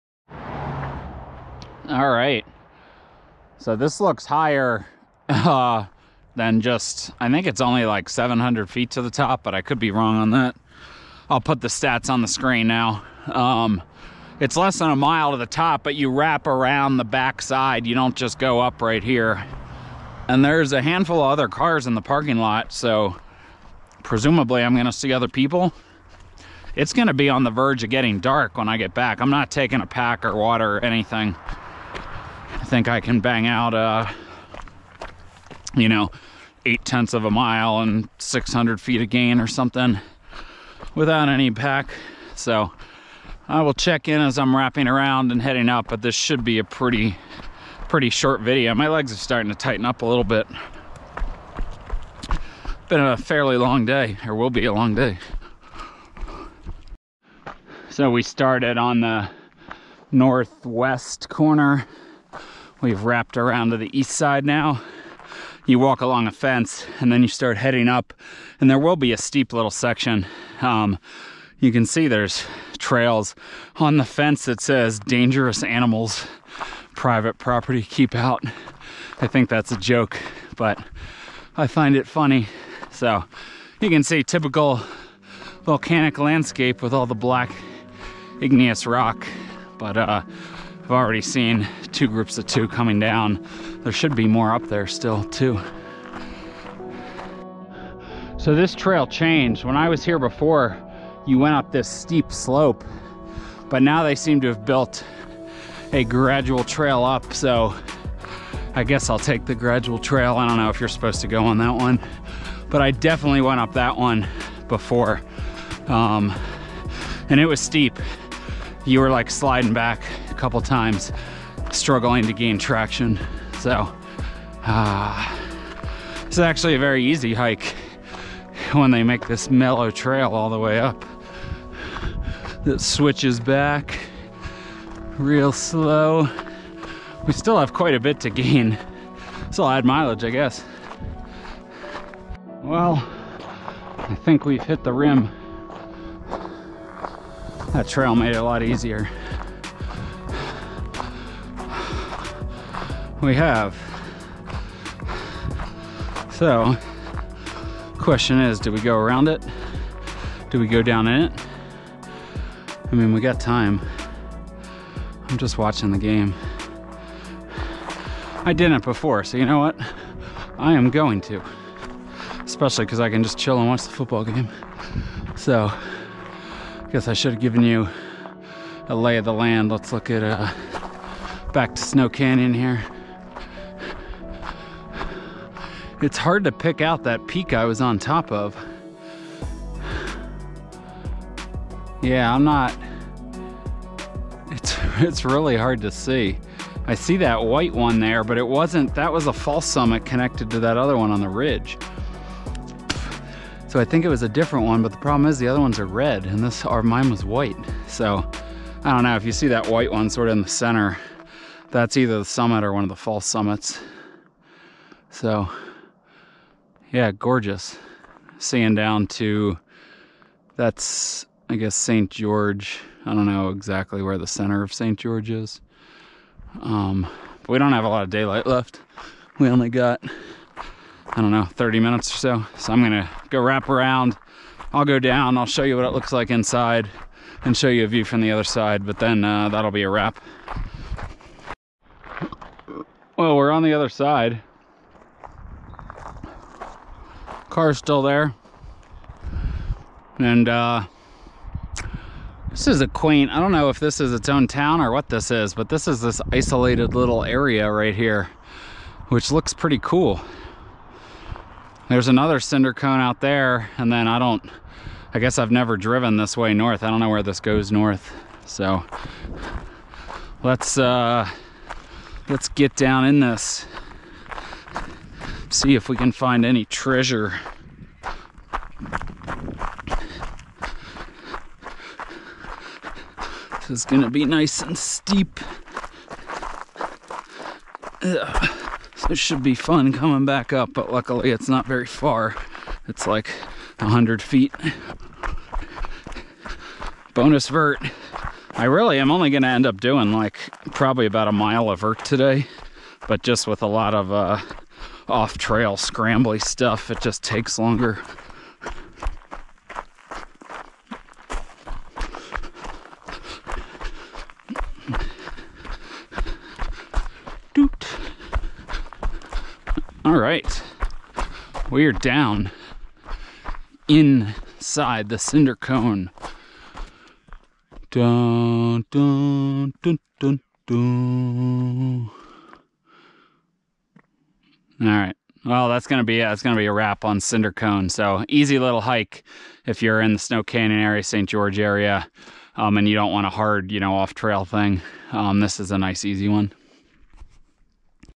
Cinder Cone - Snow Canyon State Park, St George, UT
Cinder Cone is a very easy short hike to the top of a cool cinder cone volcano, located in Snow Canyon State Park in St George, UT.